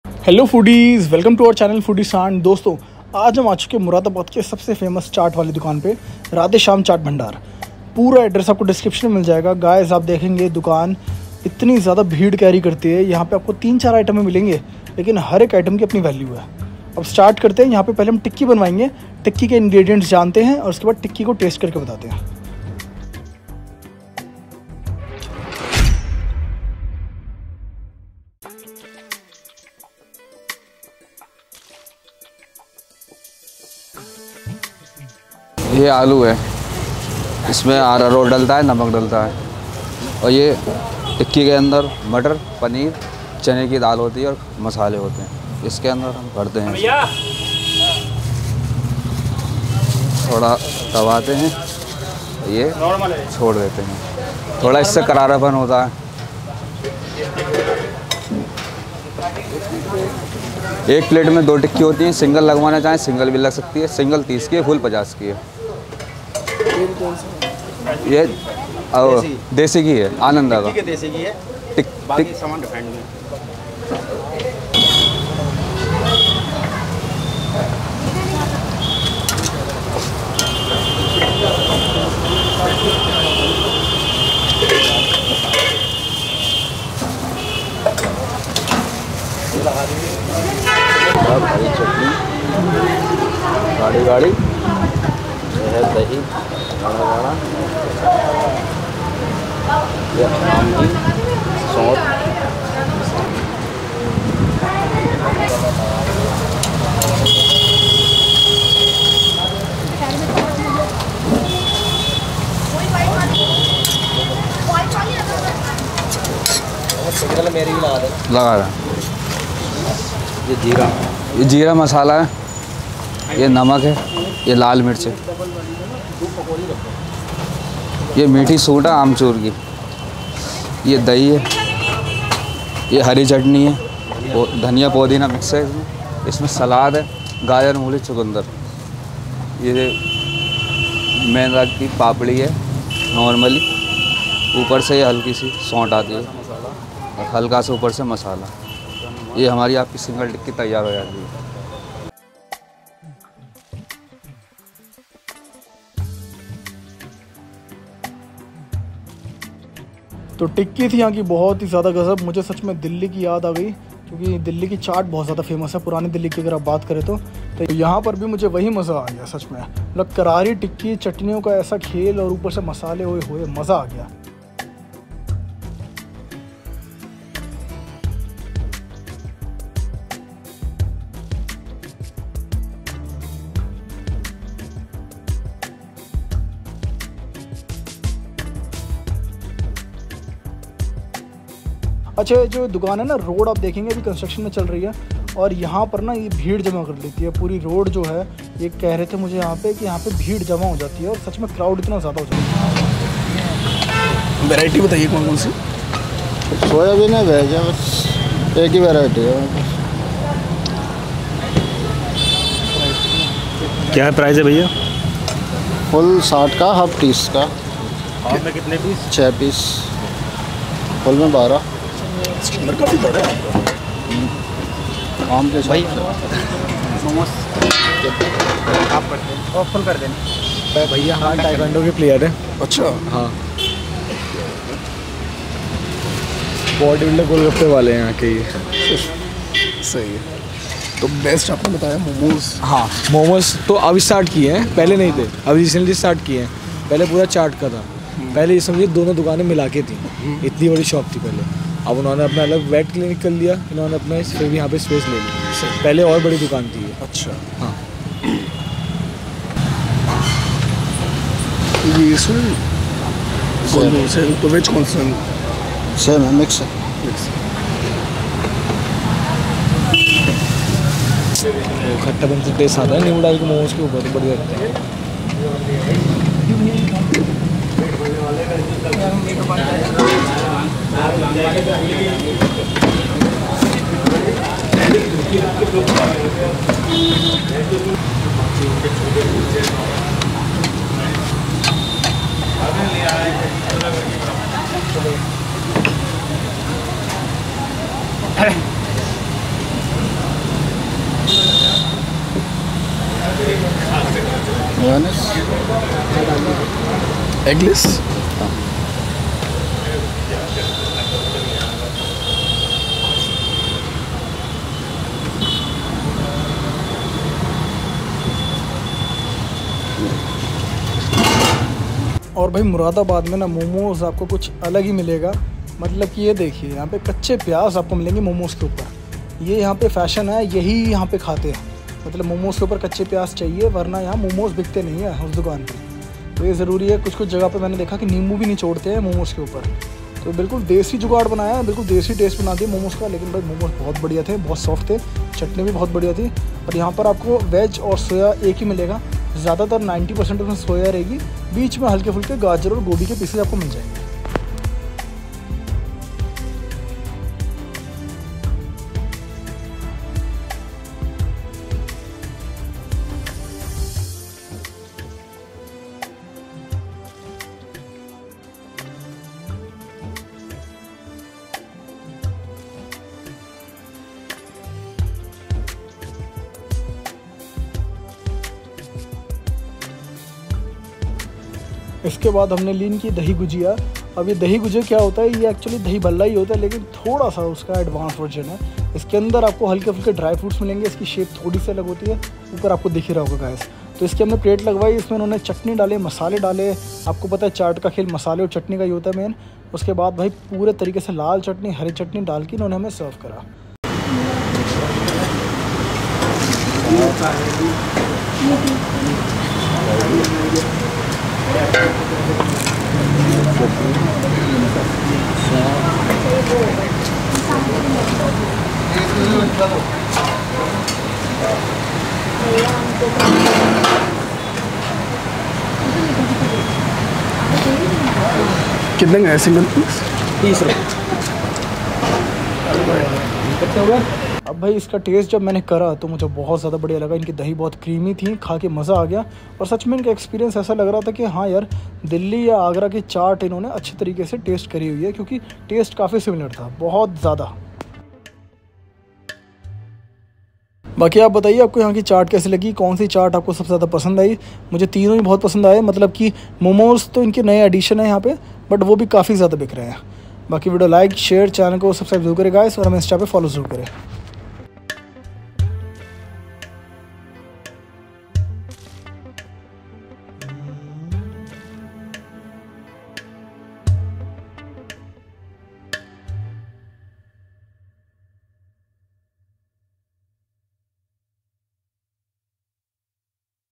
Radhe shyam Ji Ki Dahi Gujiya, Veg Soya Momos, Aloo Tikki & More || Moradabad Street Food
Opening Hours : 4pm to 11pm
Monday to Sunday (No Holiday)
Price : Aloo Tikki - 30 Rs
            Veg/Soya Momos - 30 Rs
            Dahi Gujiya - 30 Rs
Mumbai Series :
YouTube Gears :
2nd Mic
Phone Memory
Voiceover Mic